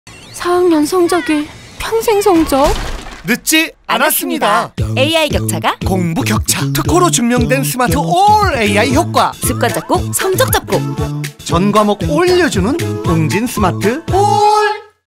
4학년 성적 평생 성적? 이제 AI격차가 공부격차! 웅진 스마트올! 신동엽, 소이현이 추천해요 [웅진씽크빅 TV-CF 4학년 성적 편]
AI격차가 공부격차!
습관잡고 성적잡는
AI교육특허 1위 초등학습
웅진 스마트올!

1:1 인공지능이 아이실력에 딱 맞는
개인 맞춤학습을 제공해
정답률 10.6%, 학습완료율 14% UP!
이제, 결과로 증명된 스마트올AI를 만나보세요

전과목 올↑려주는 스마트올↑
0원 무료 신청 / 더 알아보기